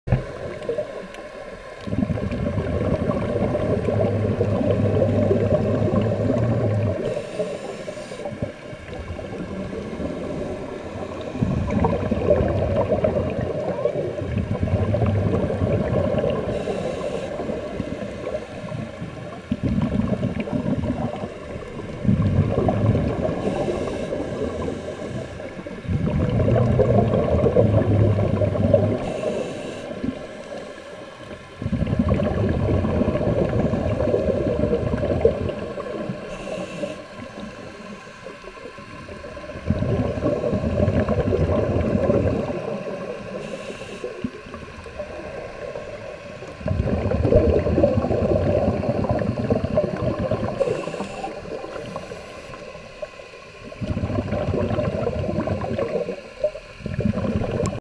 Swimming with the Turtles
Watch as the dive buddies all gather around and film the turtles of Bonaire. We shot this video in the summer of 2019 in Bonaire. We are in about ten feet of water.